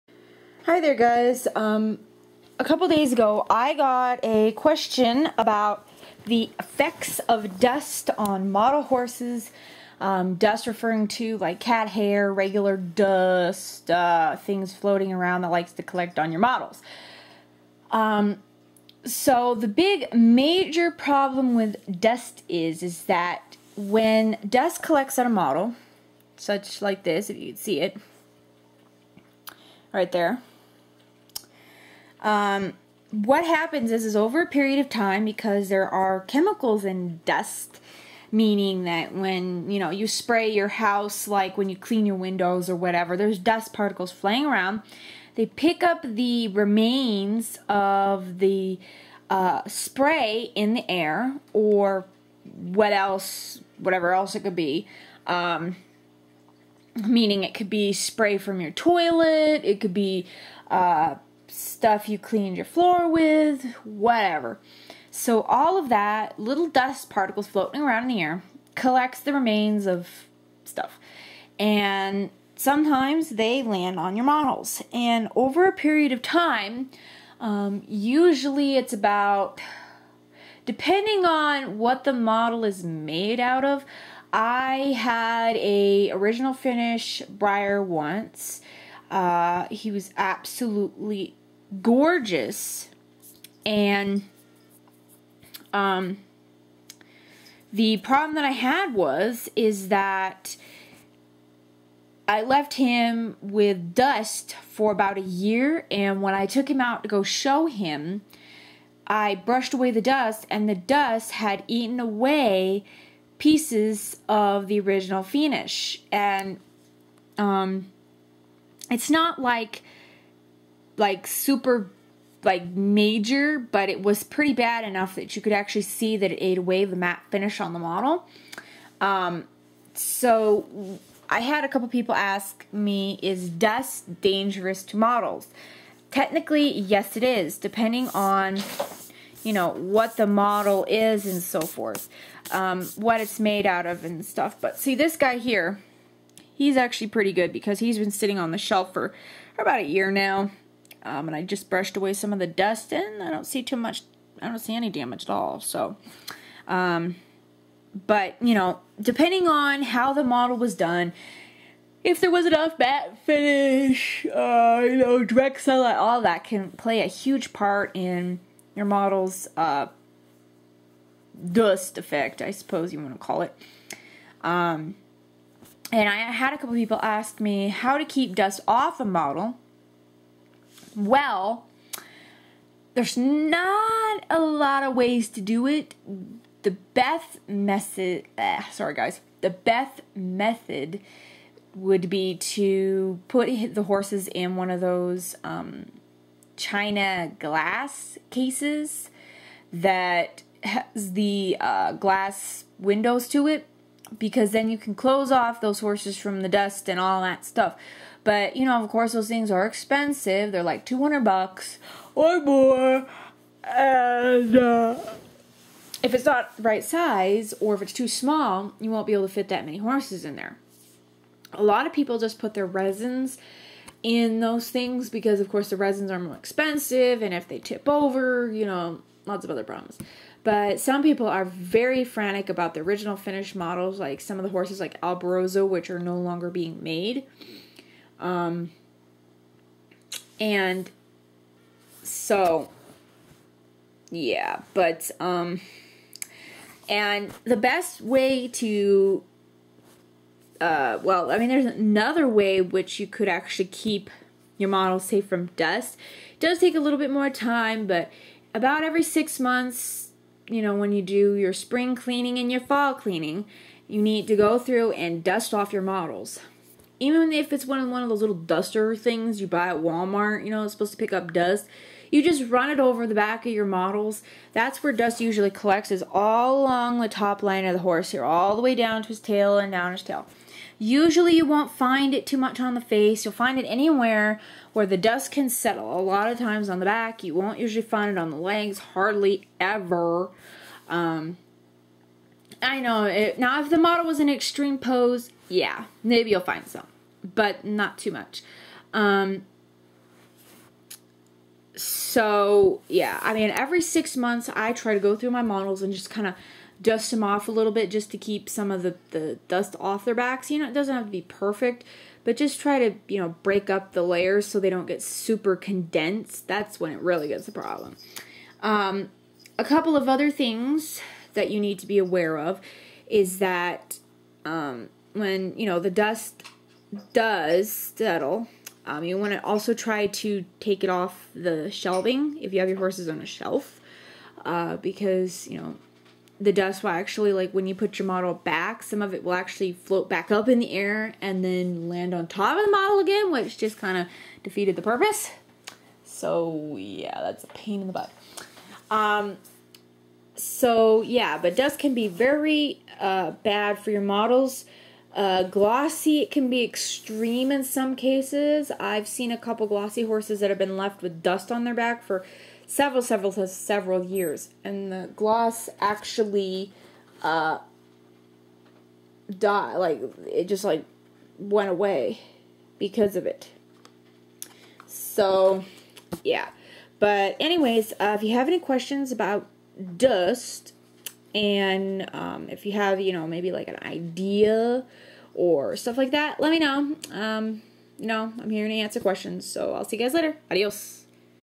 Model Horse Tutorial-Evil Dust
What dust can do to a model left to long with it on.